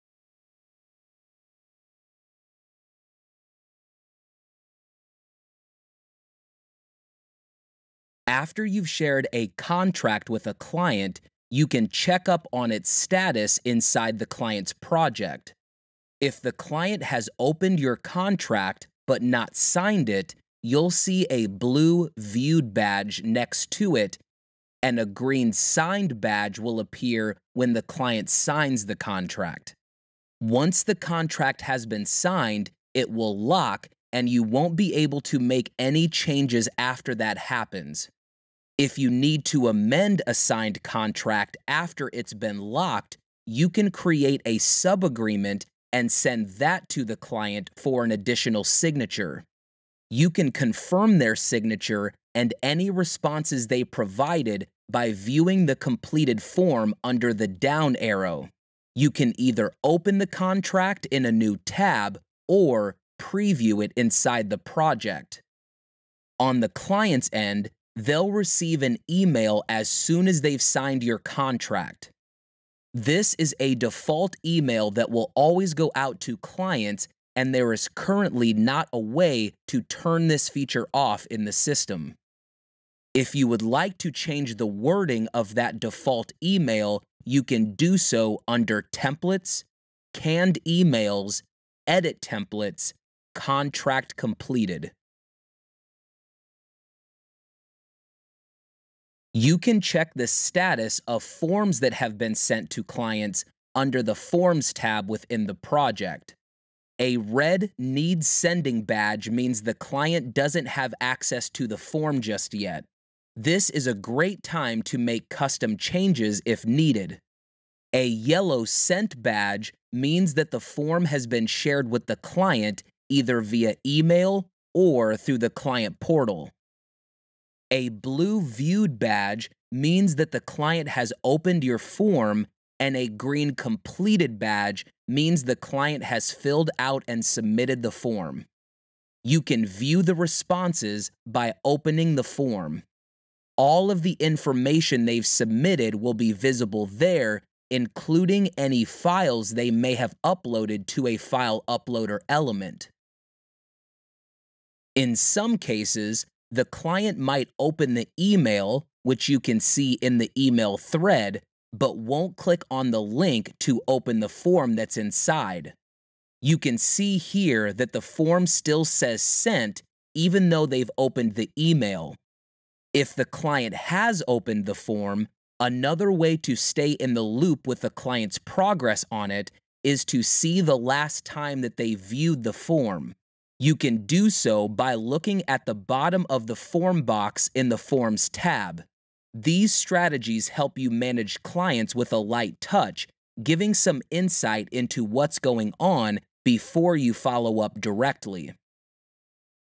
Create with Forms / Lesson 7: Checking Client Responses & Progress Badges
**Contracts**

After you've shared a contract with a client, you can check up on its status inside the client's project.

If the client has opened your contract but not signed it, you'll see a blue "Viewed" badge next to it and a green "Signed" badge will appear when the client signs the contract. Once the contract has been signed, it will lock and you won't be able to make any changes after that happens. If you need to amend a signed contract after it's been locked, you can create a sub-agreement and send that to the client for an additional signature.

You can confirm their signature, and any responses they provided, by viewing the completed form under the down arrow. You can either "Open" the contract in a new tab or "Preview" it inside the project.

On the client's end, they'll receive an email as soon as they've signed your contract. This a default email that will always go out to clients and there is currently not a way to turn this feature off in the system. If you would like to change the wording of that default email, you can do so under Templates → Canned Emails → Edit Templates → Contract Completed.

**Questionnaires or Sub-Agreements**

You can check the status of forms that have been sent to clients under the “Forms” tab within the project.

A red "Needs Sending" badge means the client doesn't have access to the form just yet. This is a great time to make custom changes, if needed.

A yellow "Sent" badge means that the form has been shared with the client, either via email or through the client portal.

A blue "Viewed" badge means that the client has opened your form and a green "Completed" badge means the client has filled out and submitted the form. You can view the responses by opening the form. All of the information they've submitted will be visible there, including any files they may have uploaded to a File Uploader element.

In some cases, the client might open the email (which you can see in the email thread), but won't click on the link to open the form that's inside. You can see here that the form still says “Sent”, even though they’ve opened the email.

If the client has opened the form, another way to stay in the loop with a client’s progress on it, is to see the last time that they viewed the form. You can do so, by looking at the bottom of the form box in the Forms tab. These strategies help you manage clients with a light touch, giving some insight into what's going on before you follow up directly.